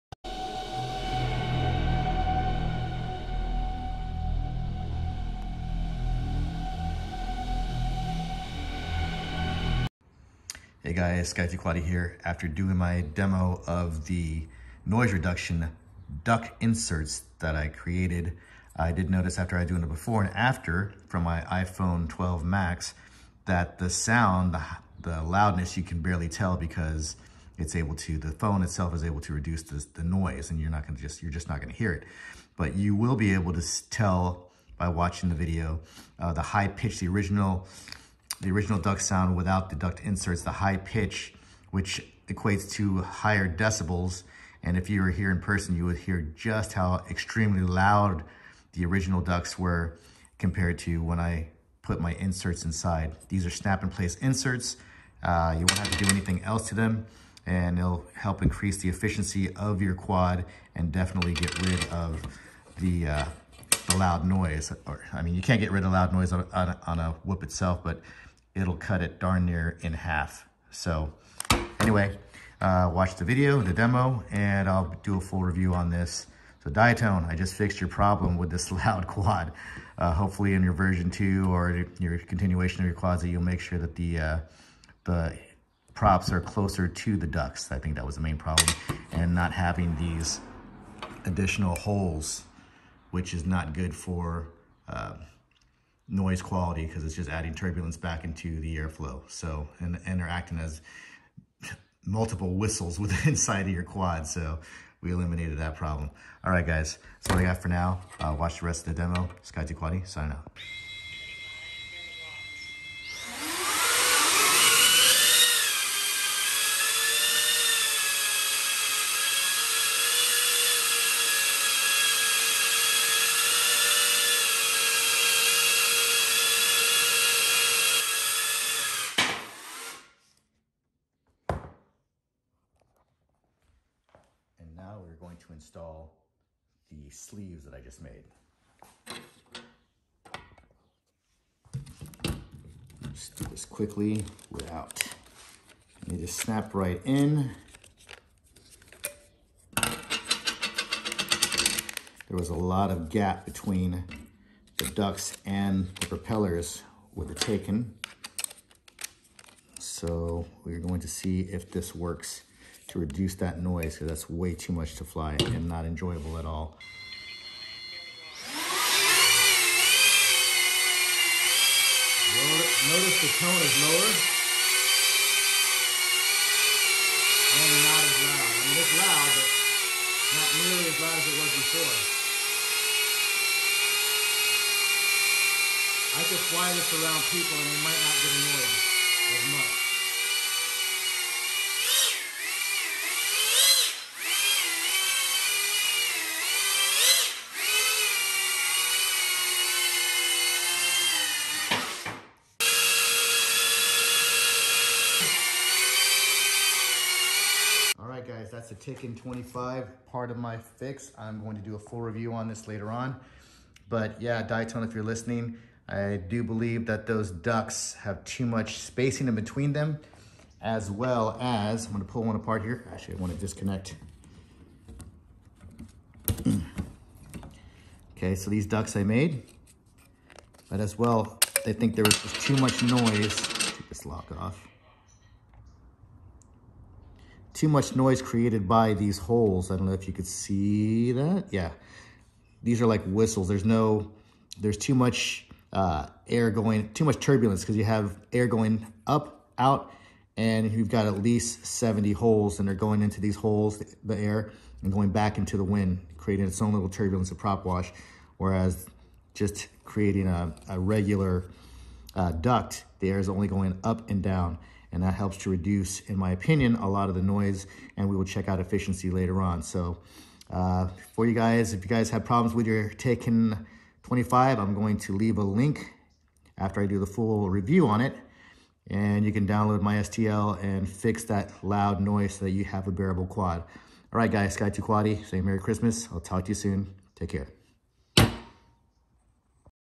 Diatone Taycan 25 Loud Cinewhoop Noise Problem FIXED! Longer Flight Times - 3D .STL Download
EDiT = I'm getting an upwards of 2 minutes more flight time with the my ducts using 850 mah 4s lipos. I was averaging 2.5 to 3 minutes prior with an action camera. Now my click on Ducts are averaging 4.5min~5min Flight times with and 850 mah 4s battery...And I'm running a heavier Caddx Vista unit

Hey Guys,

I will be doing a full review on the Diatone Taycan 25 soon. I purchased the Vista version and had some challenges I will share. But the MAIN issue is that the Diatone Taycan25 has got to be the most High Pitch noisiest Cinewhoop on the market. You will not believe how loud it is till you actually own one and fly it in real life. Anyway, watch the video and you will notice the reduction in pitch and noise from my modification. I know others will copy my idea and claim it as theirs, but as long as it works for your situation, it's all good.

Below is a link to my first Thingiverse Creation which is a noise reduction duct insert that you can easily pop in and out without modification to the Diatone Taycan 25 Cinewhoop. I should also help increase efficiency as well as thrust

Scotty2Quady Noise Reduction Duct Insert